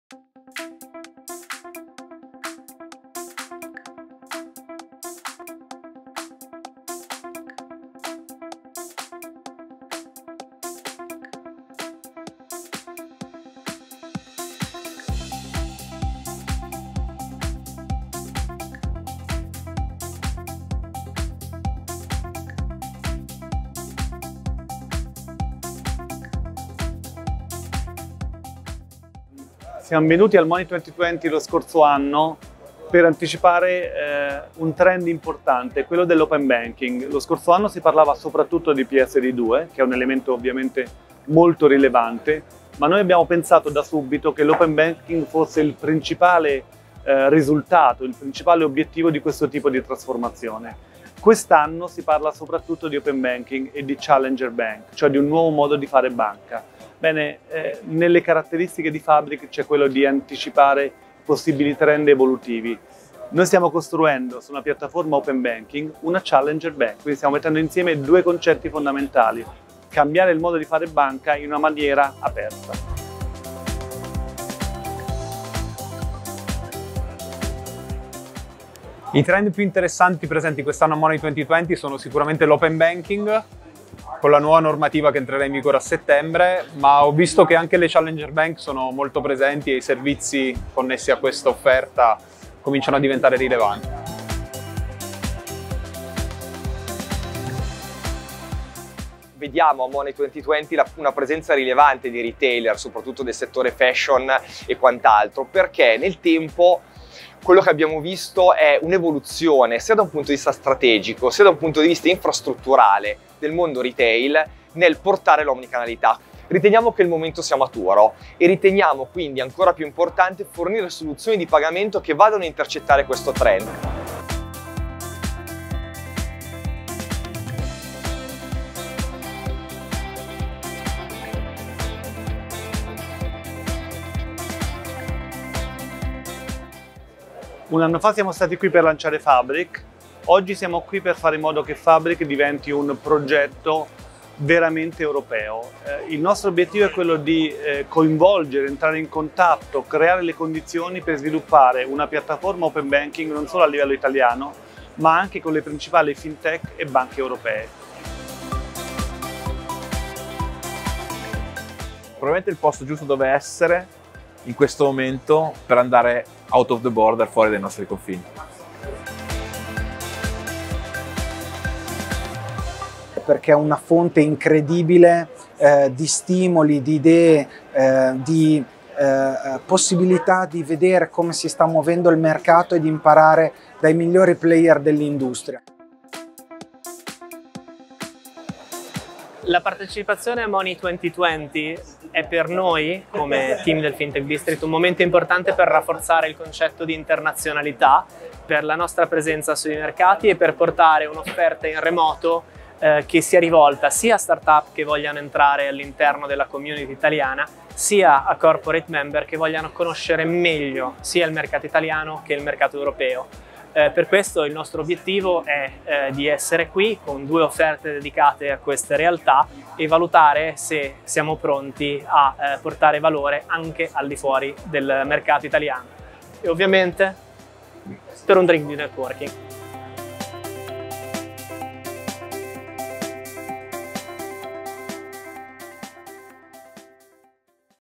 ​Money 20/20 Europe 2019 - Fabrick's highlights
Fabrick was among the protagonists of Money 20/20 Europe again this year. Find out with what expectations he has participated and what news he has presented.